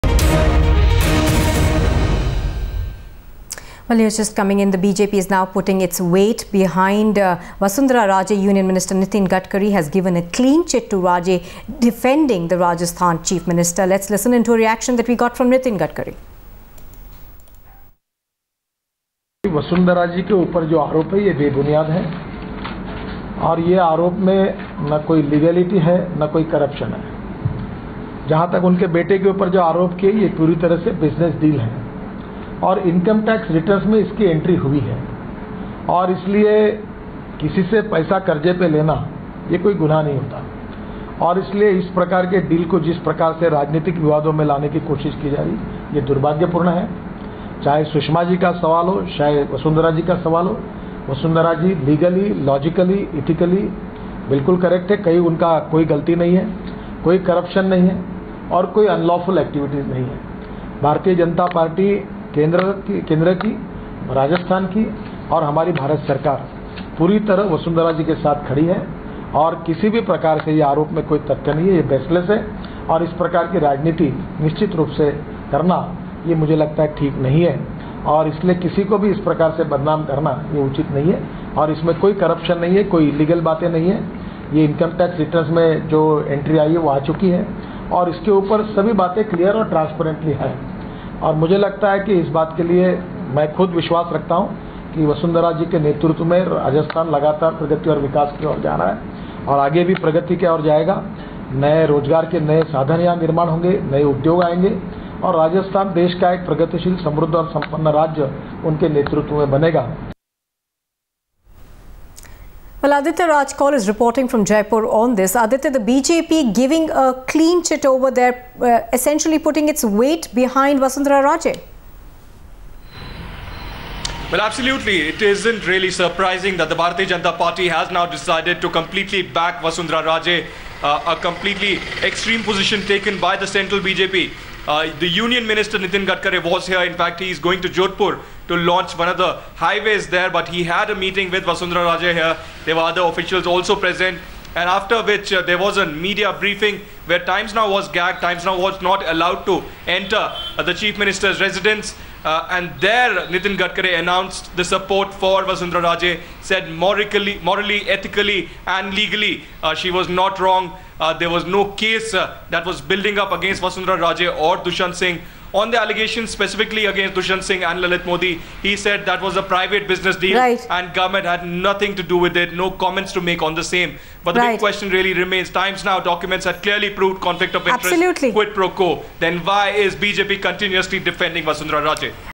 Nitin Gadkari gives clean chit to Vasundhara Raje
Union Minister Nitin Gadkari today came out in strong defence of Rajasthan Chief Minister Vasundhara Raje, asserting that Central government and BJP stands with her. "Vasundhara Raje has done nothing wrong morally, legally and ethically. All allegations against her are baseless," he said. Doing this kind of politics is not right," Gadkari said, adding "the state and the government is standing behind Vasundhara fully". His statement came after his meeting with Raje in Jaipur today amid the raging row over the linkages of the Chief Minister and her son with Lalit Modi who is facing ED probe for alleged money laundering.